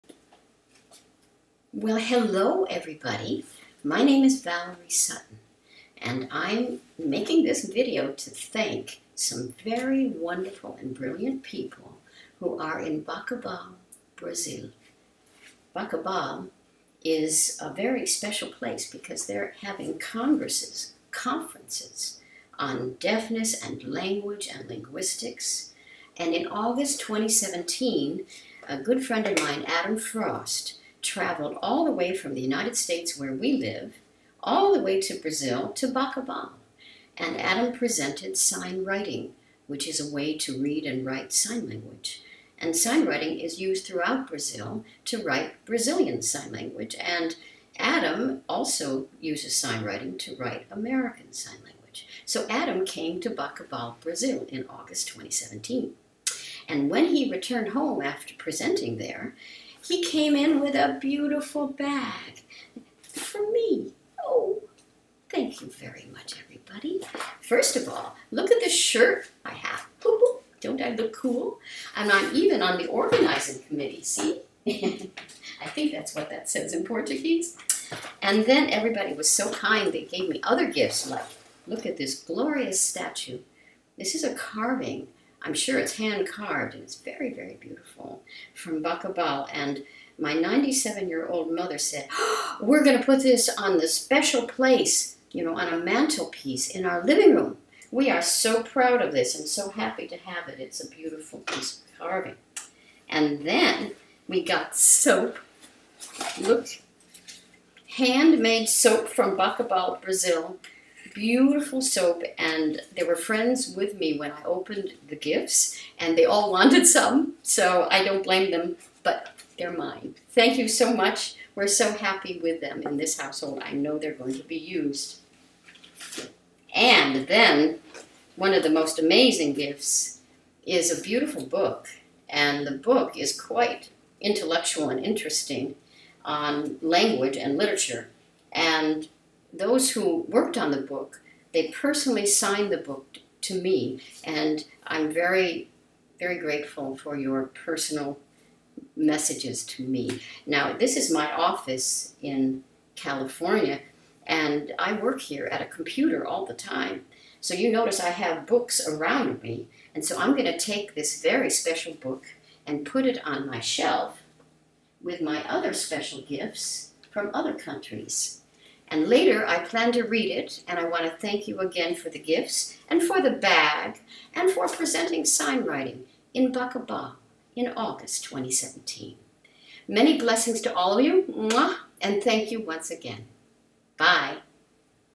Valerie Sutton Thanks the Brazilian Congress in Bacabal August 2017
Valerie Sutton thanks Professor Gérison Kézio and the Organizing Committee of the I Congresso Internacional de Letras (I CONIL) "Lingua Portuguesa e suas Literaturas no Mundo" held in Bacabal, Maranhão, Brazil, in August 2017, for the many gifts they gave ;-)